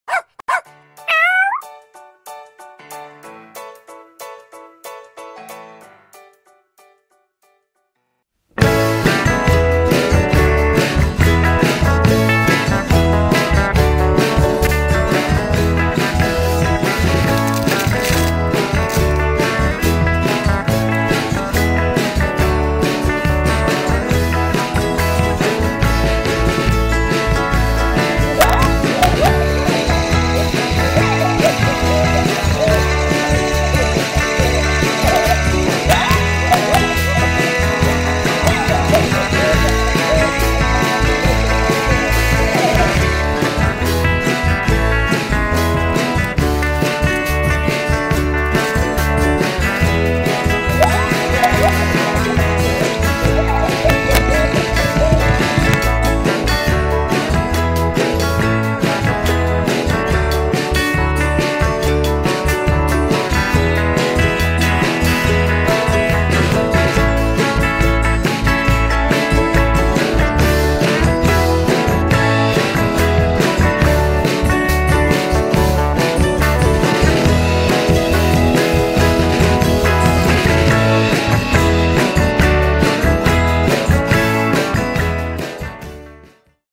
COOCKOD Bouncy interaktywna zabawka dla psa, niebieska
Zabawka Coockoo Bouncy zachęci do zabawy nawet najbardziej leniwe psy. Włącz zabawkę a zobaczysz, że Twój pies nie będzie chciał się od niej oderwać.